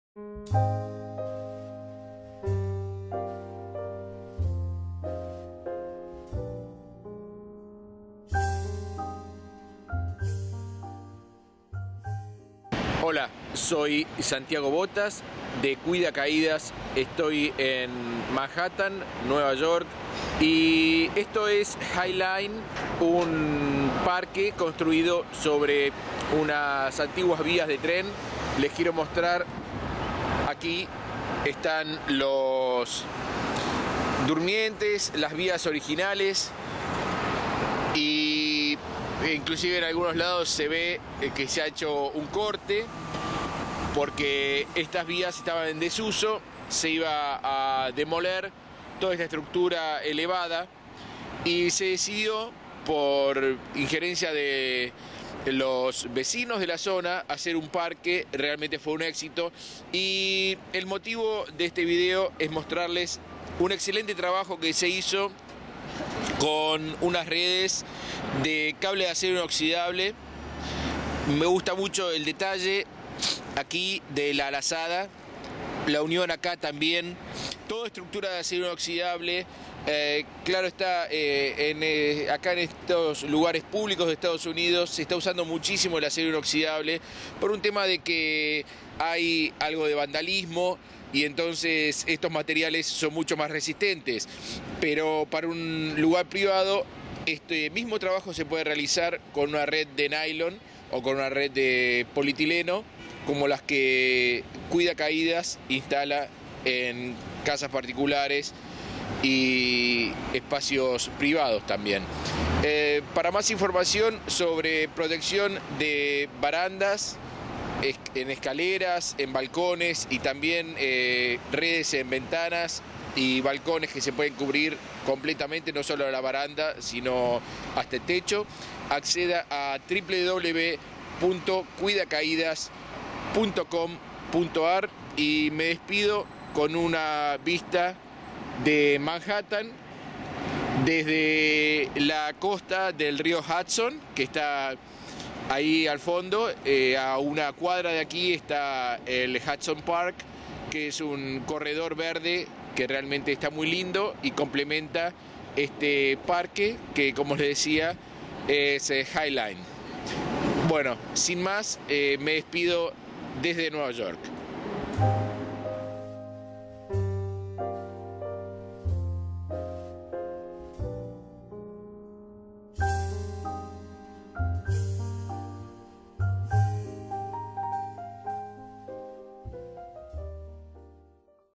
Protección de barandas con redes en Manhattan
En New York se construyó un parque elevado.
Para evitar accidentes de caídas de perros, gatos y otras mascotas y principalmente  bebés y niños se colocó una red de seguridad en las barandas.